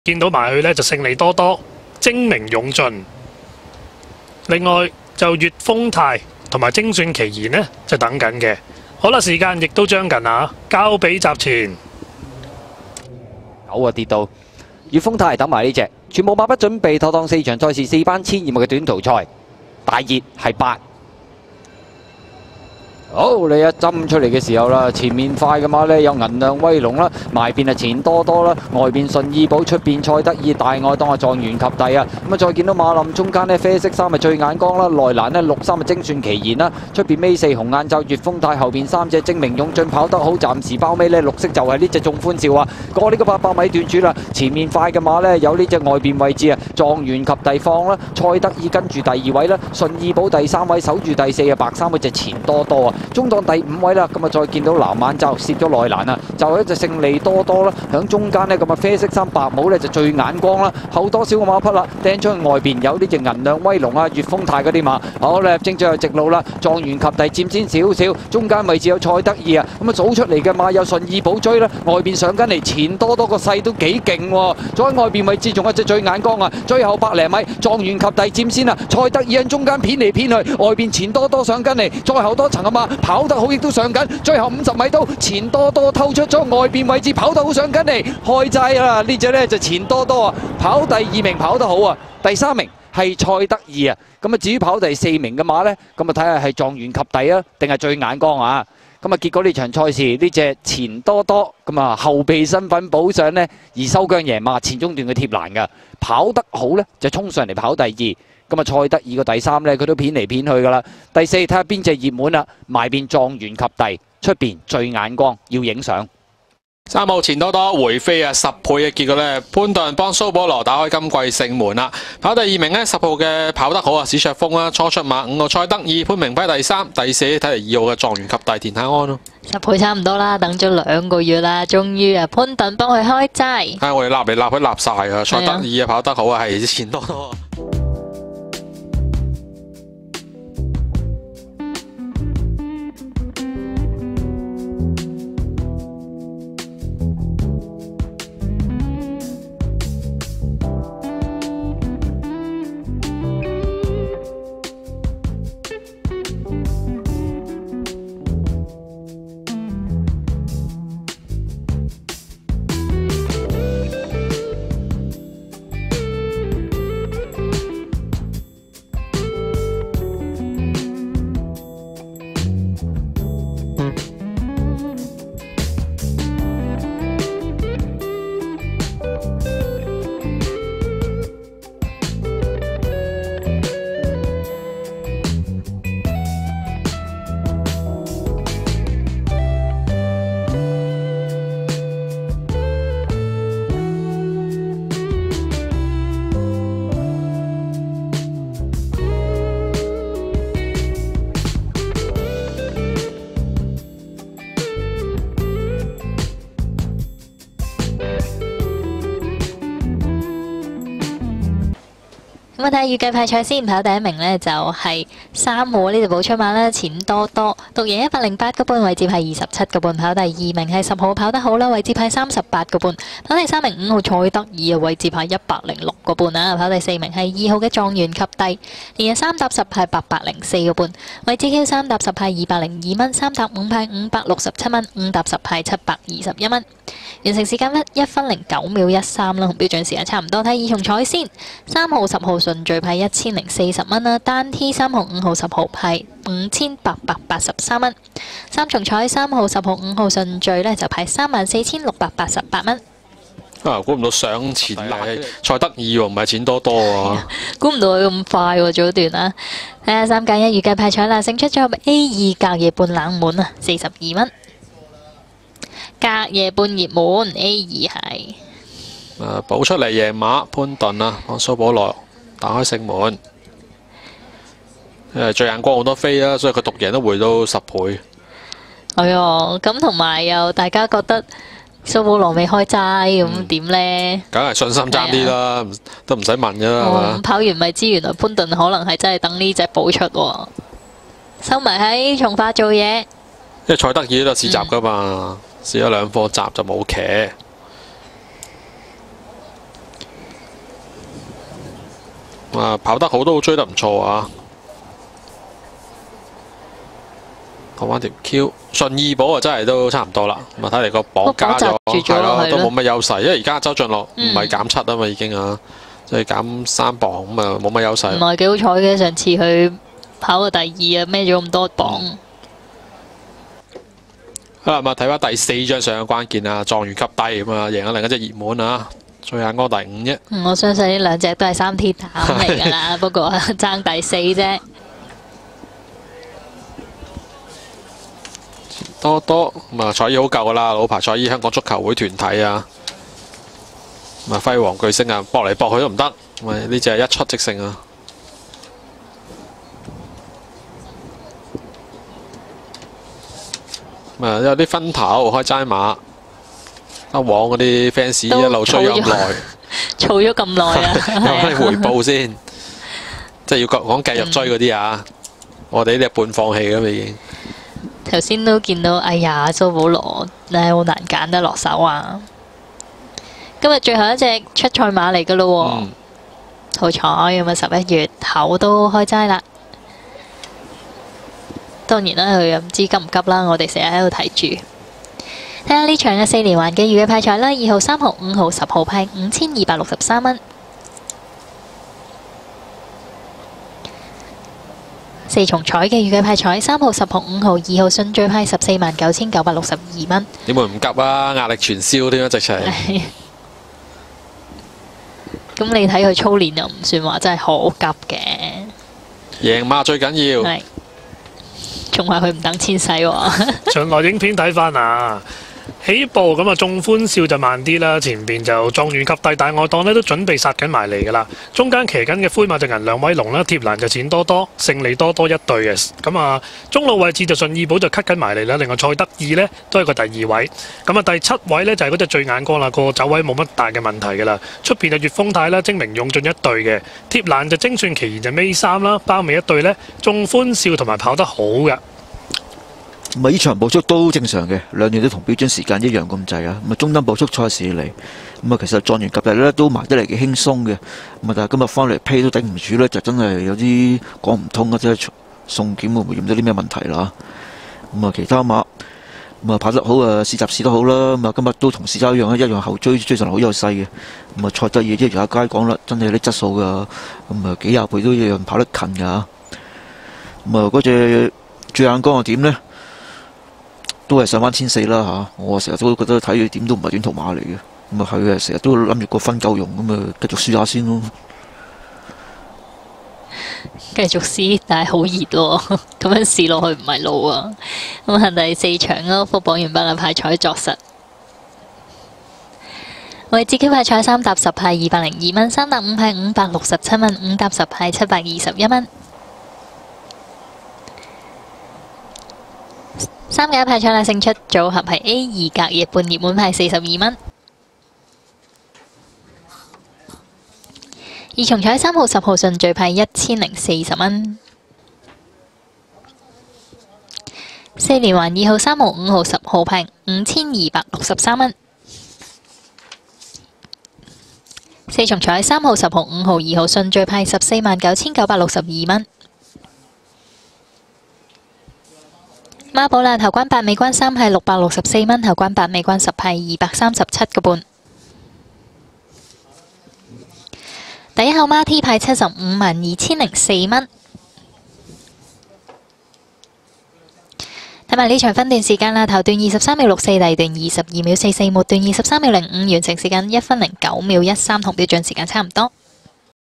2021/22 (152) - Snowalot (錢多多) - 潘頓 (對手弱初出即勝)
Jockey: Z Purton (潘頓)
Trainer: P O'Sullivan (蘇保羅)
Owner: HK Football Club Horse Racing (香港足球會團體)

2021/11/07 - Hong Kong - Sha Tin (152)

Sire: Contributer
Dam: Snowdonia